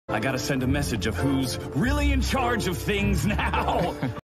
I was bored :P
I decided to draw my character over Vox from Hazbin hotel :3